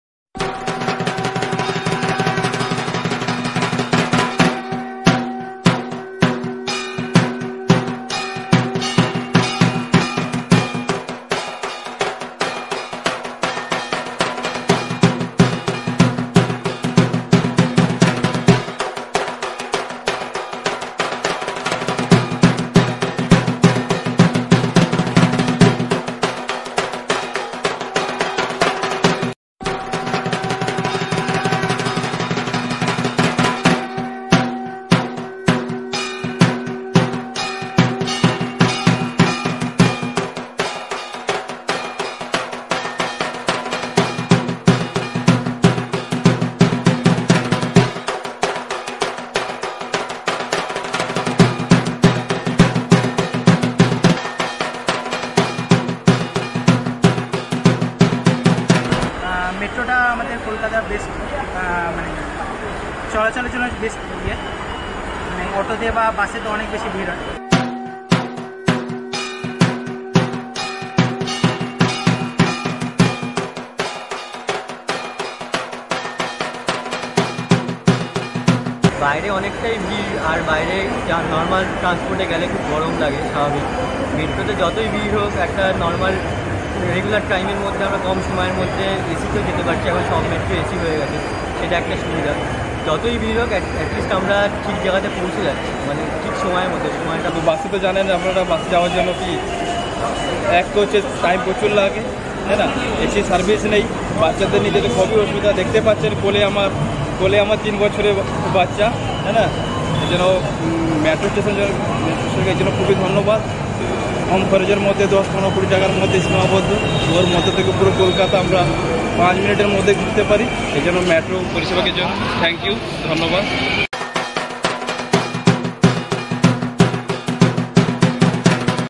On Mahanabami too, commuters took Metro to visit their favourite pandals with family and friends.
On Mahanabami too, commuters took Metro to visit their favourite pandals with family and friends. This is what they have said about our services.